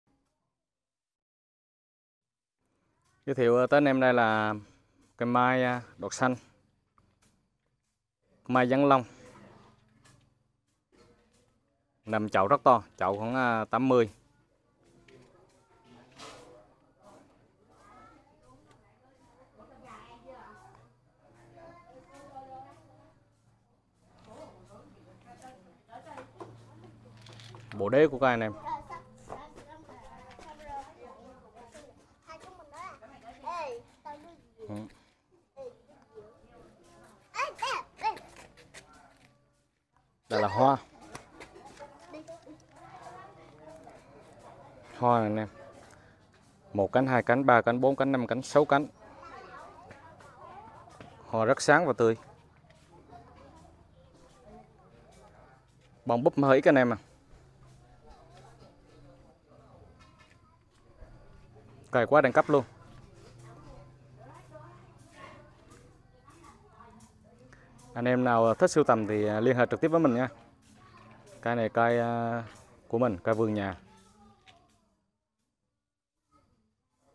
Mai đọt xanh VIP tại Bình Định | Nguyenquangbd
Tác phẩm mai đọt xanh dáng long. Xin gửi tới quý cô chú và anh chị